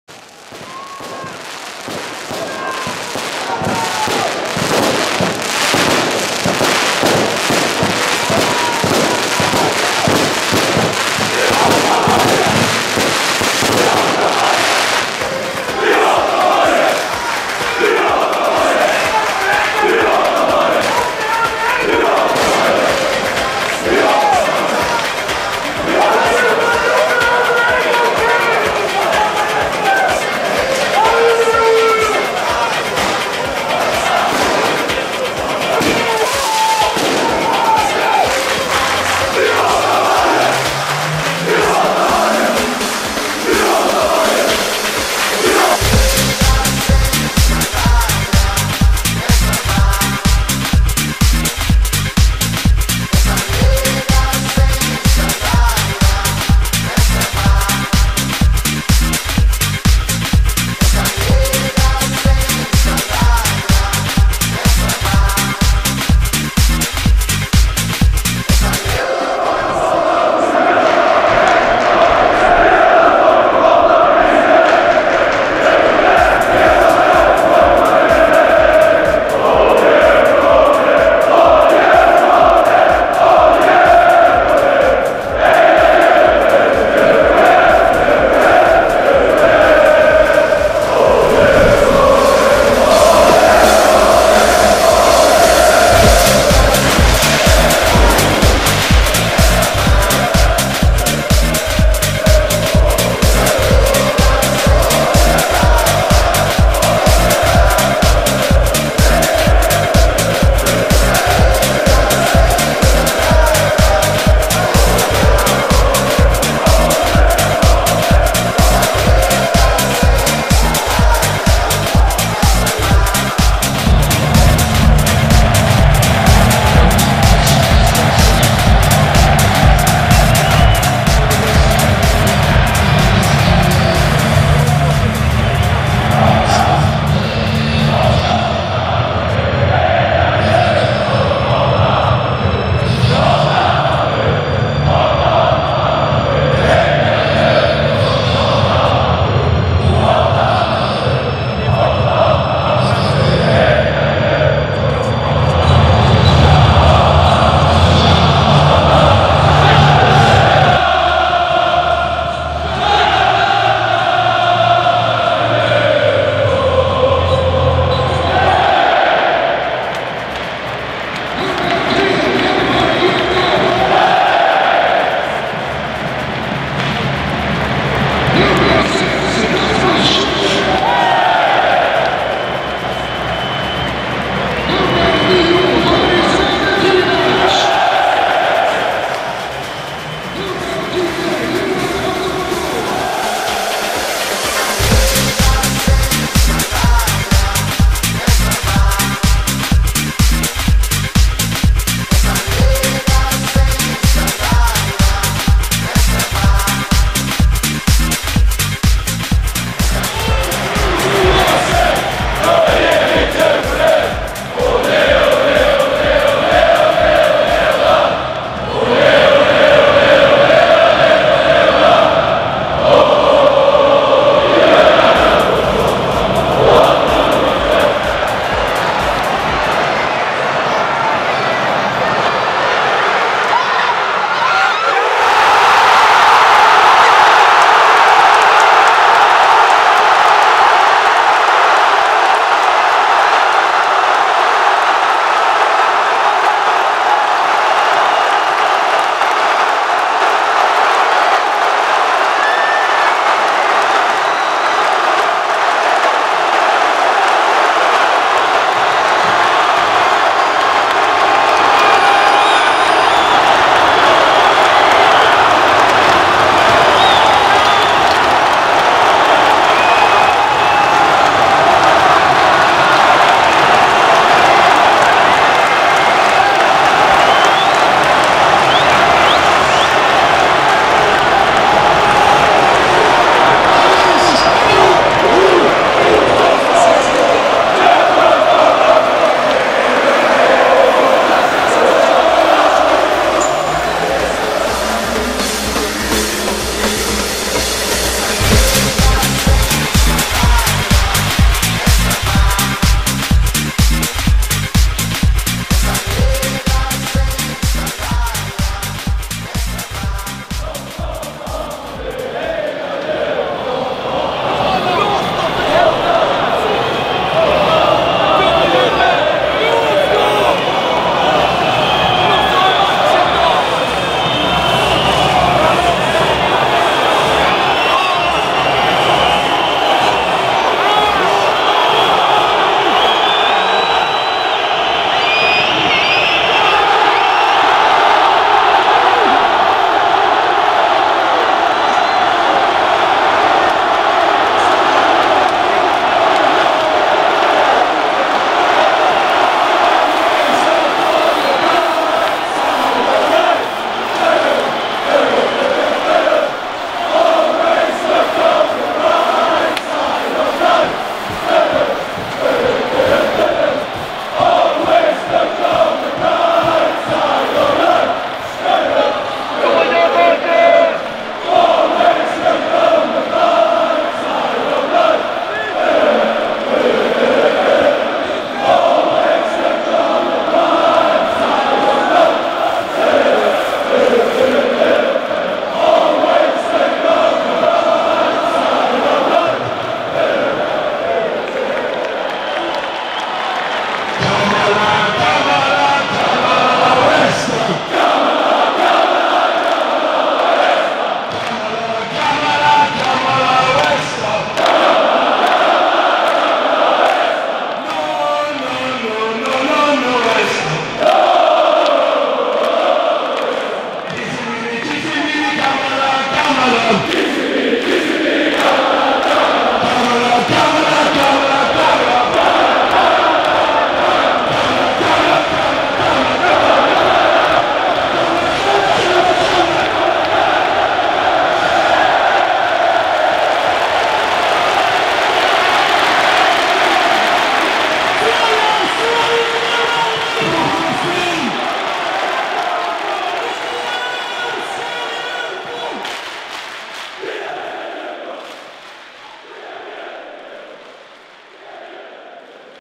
MATCHDAG | Djurgården - hammarby (1-0) (3/07 2022) Derby March Tifo
Djurgården besegrar hammarby i en toppmatch av 2022 upplaga av allsvenskan. En välbehövlig seger som innebar tre viktiga poäng i toppstriden.

Dage startar i humlegården med en episk samling där energin sedan överförs på läktaren där Sofialkäktaren levererar ett 10/10 tifo. Matchen är oviss, bajen och DIF med en del chanser men i slutändan är det DIF som gör första och ända målet. Gustav Wikheim - evig derbyhjälte.

Derby March Tifo